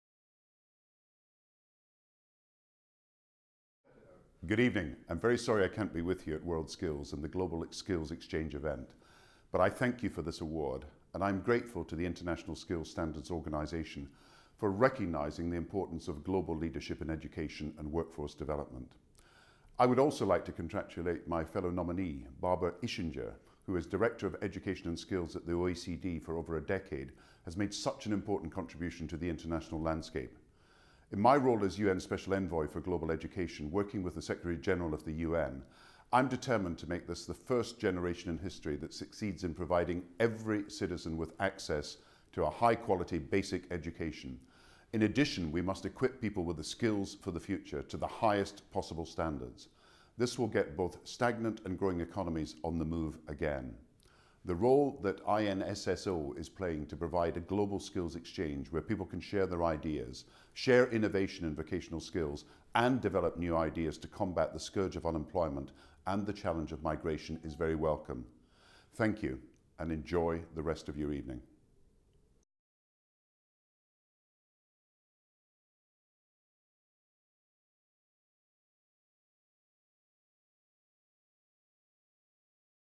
INSSO Global Award 2013: Gordon Brown (Thanks)
INSSO's Global Award for Leadership in Education and Workforce Development is handed out every two years in conjunction with our GSx Global Skills Exchange Conference.

This year (2013) saw two recipients:
Rt Hon Gordon Brown MP, UN Special Envoy for Global Education
Dr. Barbara Ischinger, Director of Education and Skills, OECD

Here, Mr Brown thanks INSSO for the award.